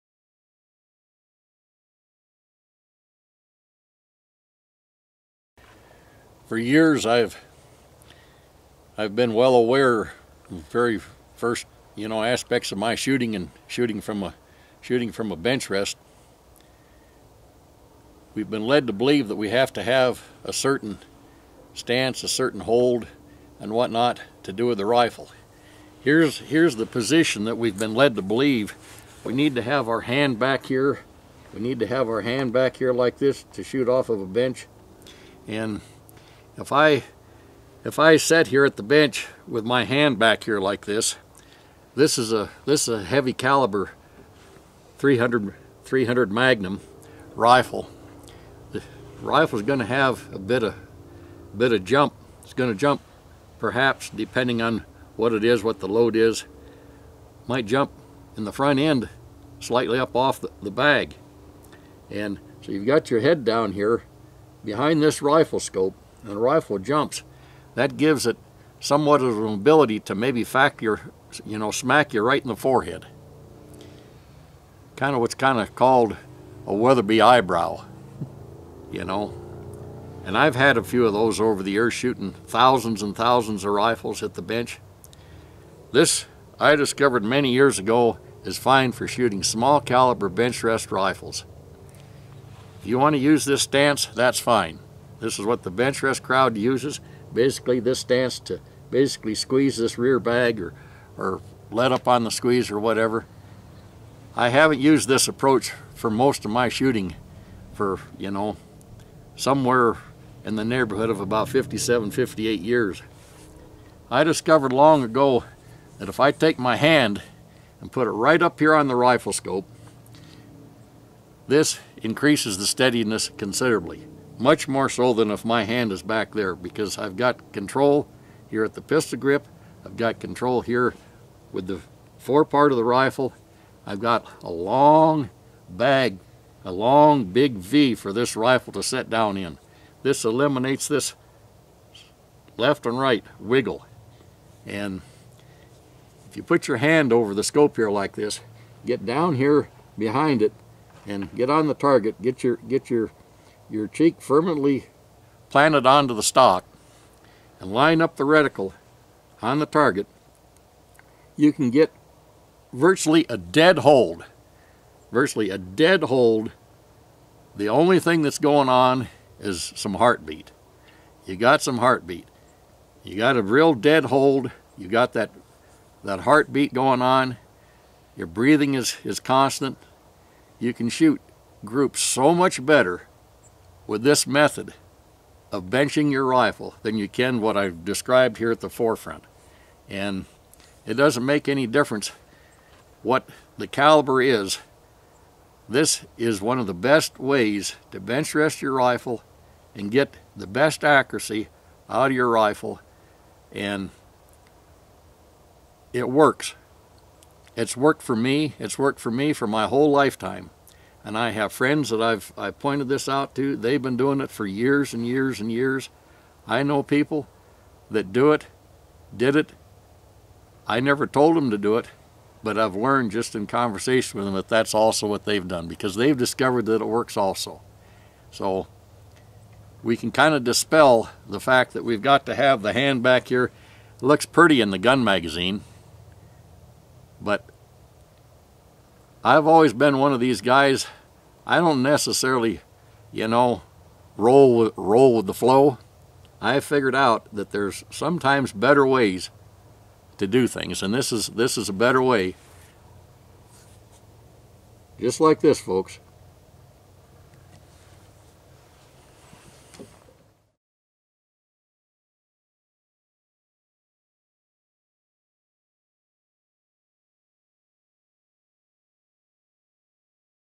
The Real Gunsmith – Bench Shooting Technique
Randy demonstrates his preferred method to shoot from the bench.